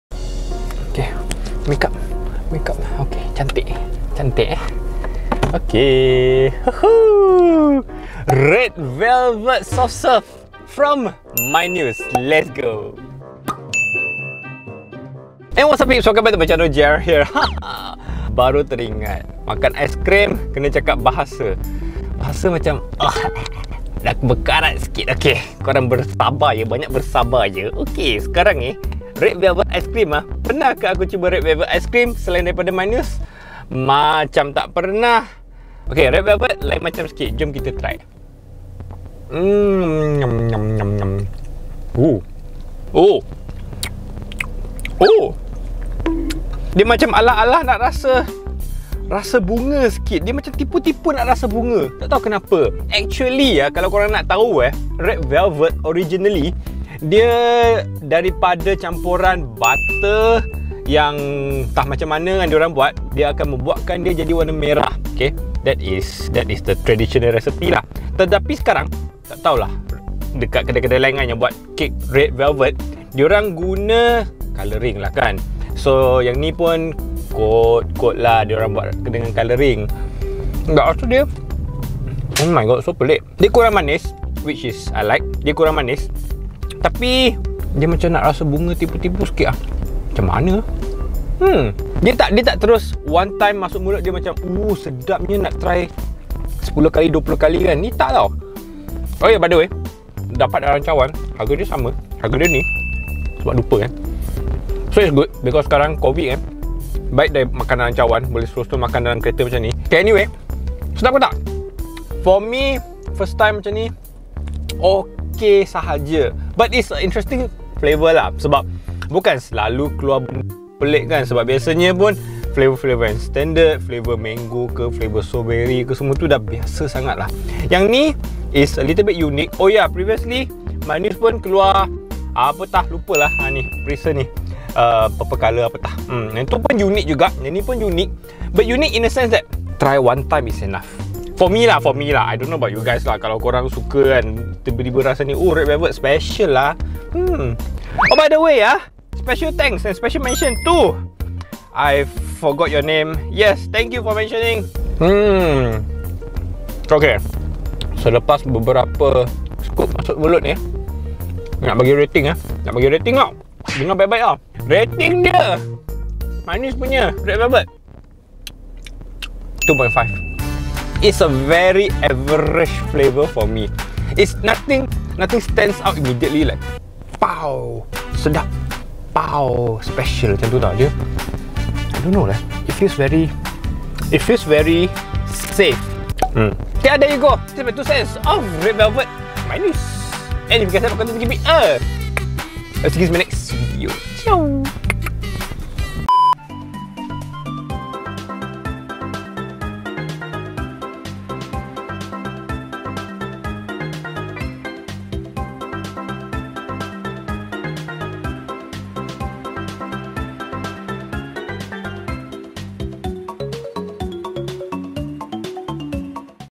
MARU RED VELVET SOFT SERVE MYNEWS | Real Red Velvet ke fake?
*BAHASA MELAYU CONTENT*
It's that time of the month, ice cream cycle of Mynews. This time is Red Velvet... Tapi. Sedap tak?